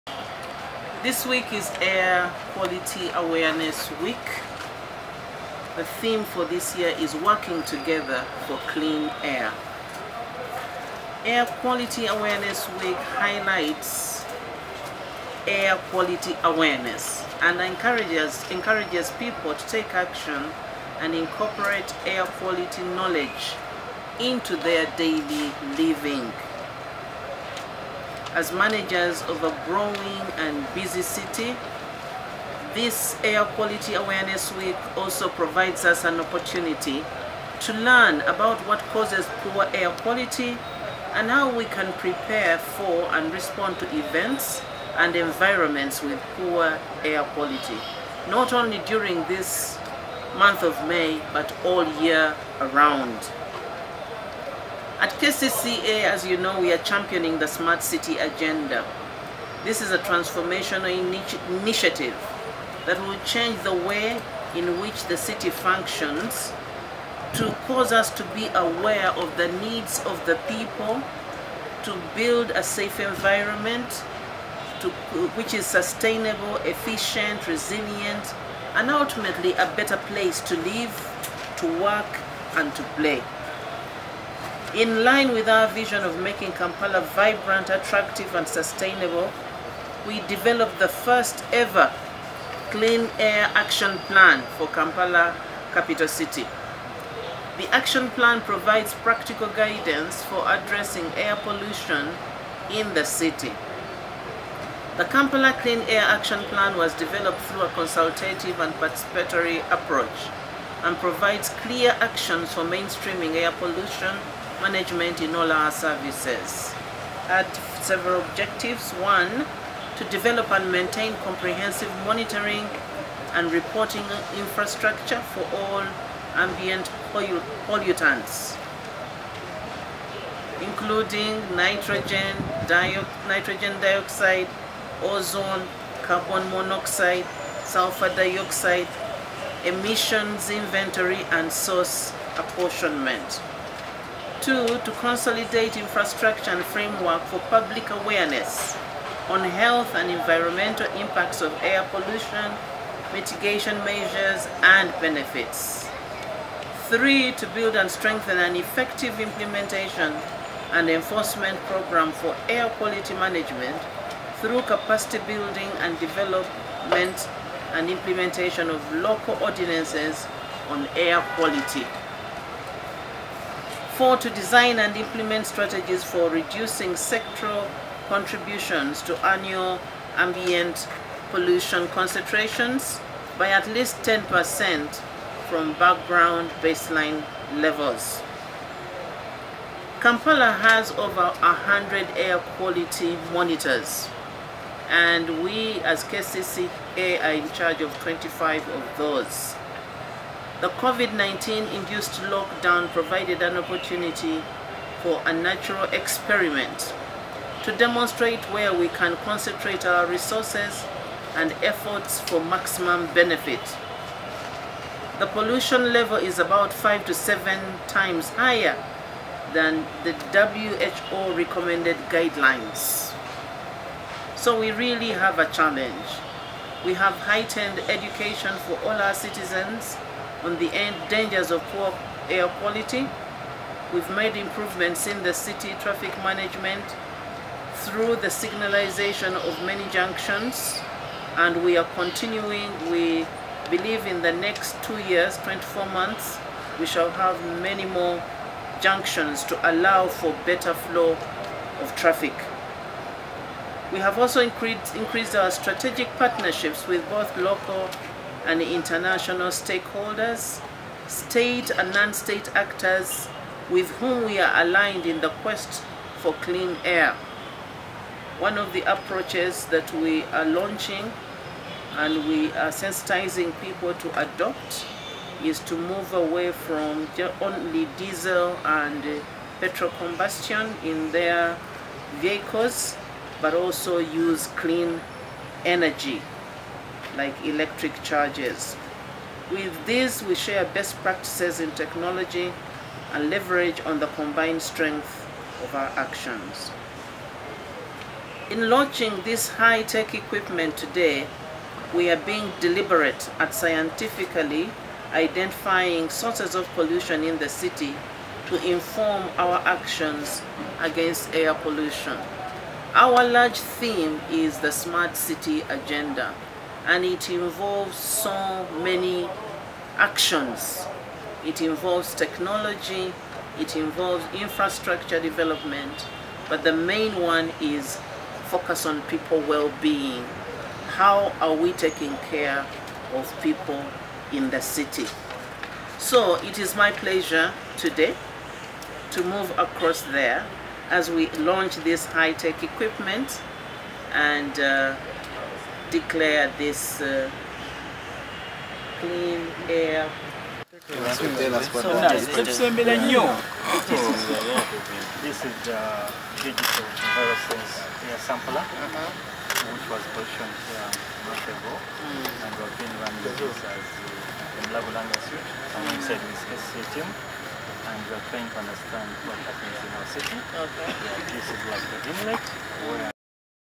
KCCA ED assures Ugandans on better Kampala as she commissions air quality monitoring machine.
KCCA ED assures Ugandans on better Kampala as she commissions air quality monitoring machine. kcca, news newslive #2019, pleasesubscribe, uneb results, school, headteacher.mulengeranews, dorothykisaka, technology, muk.